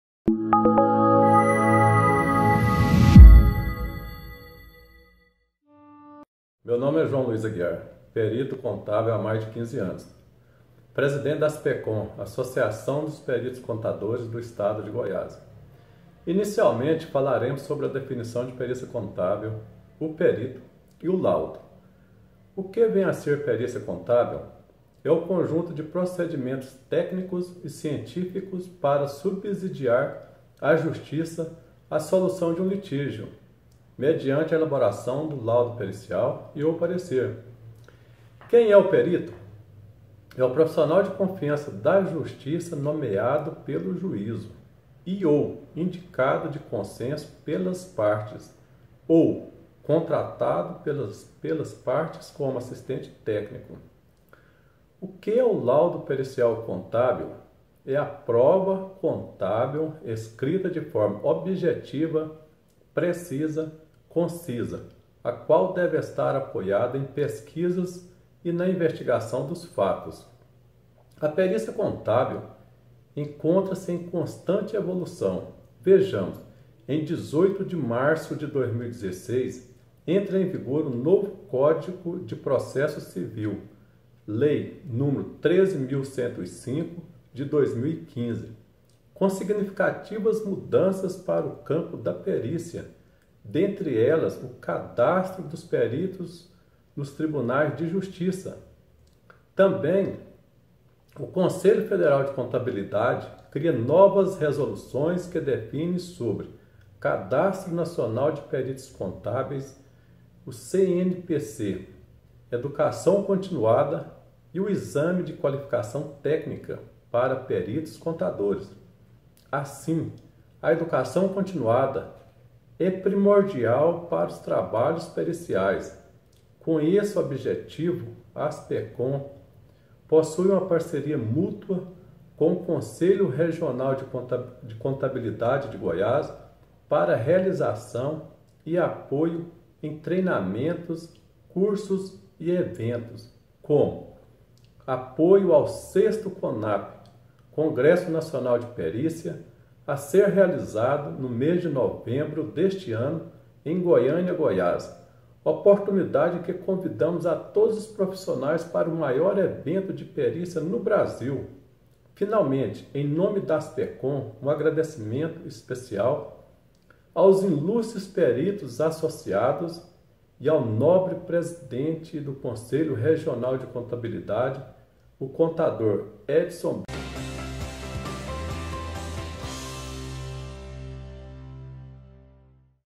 João Luis Aguiar, presidente da Aspecon
O presidente da Associação dos Peritos Contadores do Estado de Goiás (Aspecon-GO), João Luis Aguiar, em entrevista à TV CRC falou sobre a perícia contábil e a Aspecon. Confira!